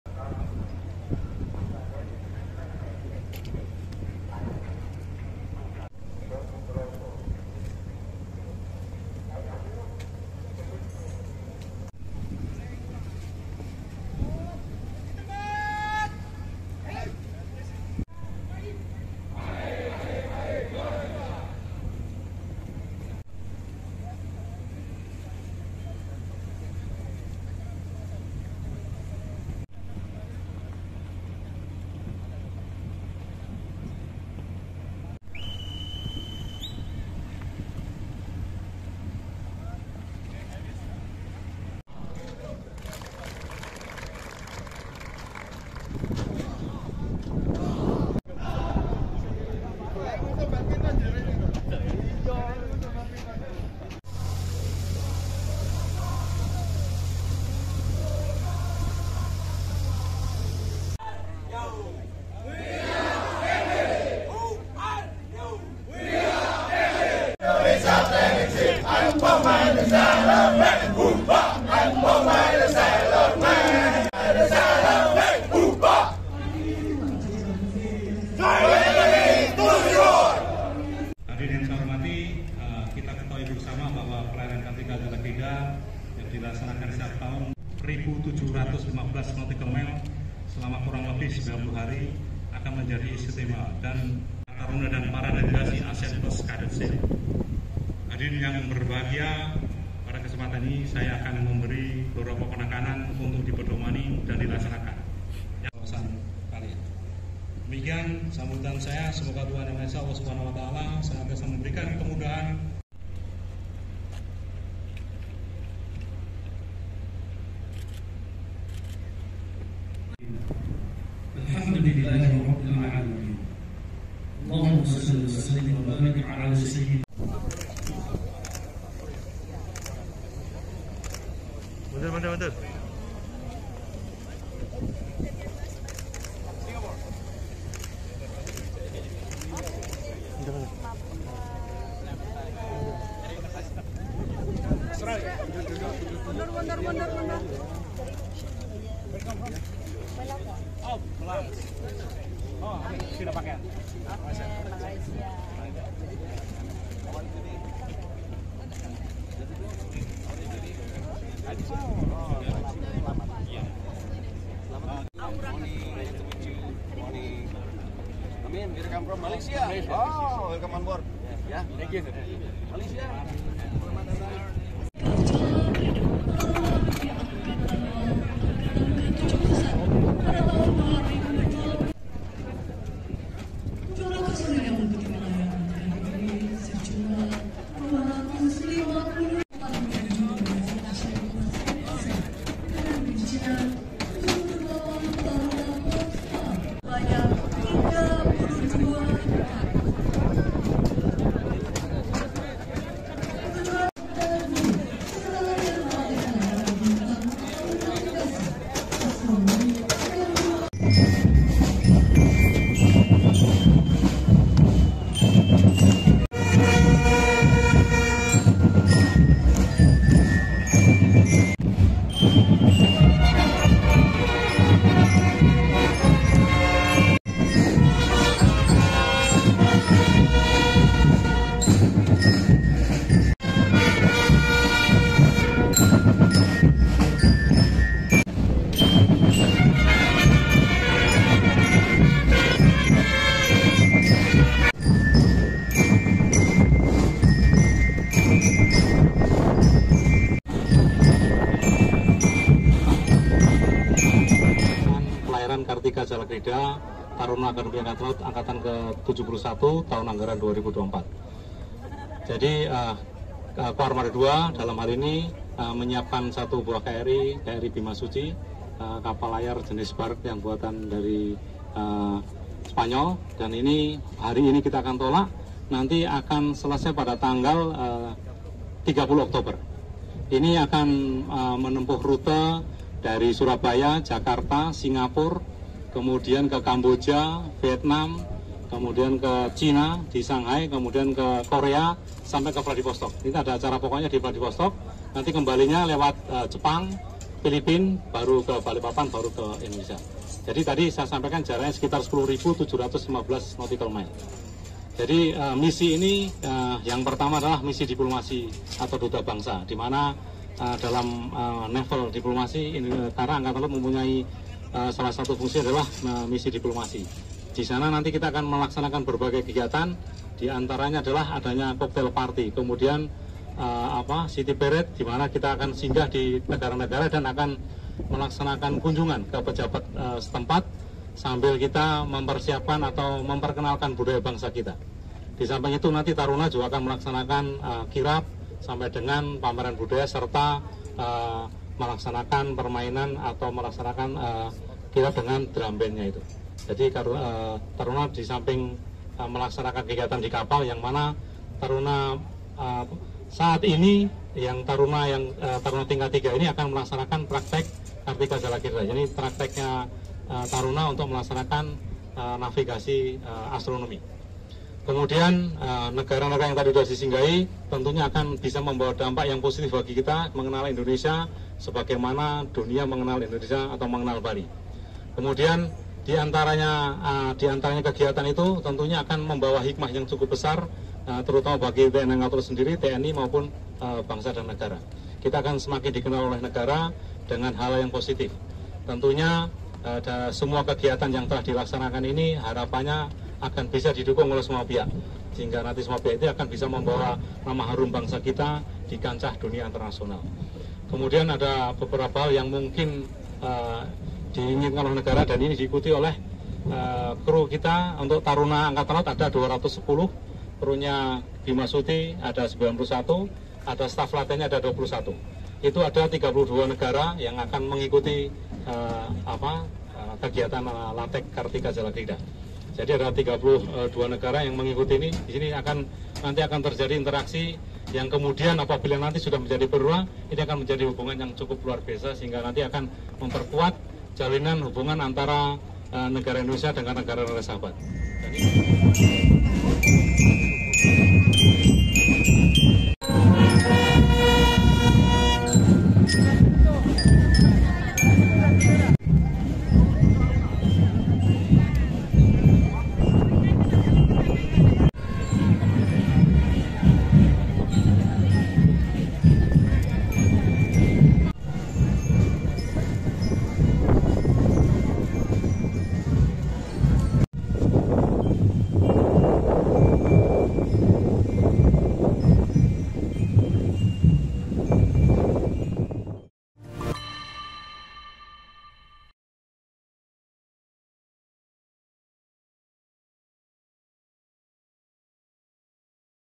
KRI Bima Suci Berlayar 90 Hari, Bawa Misi Diplomasi Jadi Duta Bangsa ke Asia hingga Rusia
Komando Armada II memberangkatkan KRI Bima Suci dalam pelayaran misi diplomasi dan muhibah dari Surabaya, Jawa Timur, Kamis (1/8/2024).

Di bawah pimpinan Letkol Laut Hastaria Dwi Prakoso, selaku Komandan Satgas, KRI Bima Suci, akan membawa personel sebanyak 91 Anak Buah Kapal, 210 Taruna AAL, serta 47 Taruna Angkatan Laut dari 32 negara sahabat.

Para Taruna AL akan mengikuti Latihan Praktek Kartika Jala Krida (Lattek KJK) Taruna Tingkat III Angkatan ke-71 Tahun 2024. Mereka akan berlayar selama 90 hari ke Asia Timur dan Rusia.

Pelayaran Kartika Jala Krida Tahun 2024 ini, merupakan ajang untuk memperkenalkan bangsa Indonesia kepada dunia internasional. Mempromosikan budaya dan pariwisata Indonesia. Selain itu, program yang diikuti 32 kadet akademi AL dari 17 negara itu bertujuan untuk membangun konektivitas antar pemuda dan calon-calon perwira TNI Angkatan Laut dengan negara-negara lain.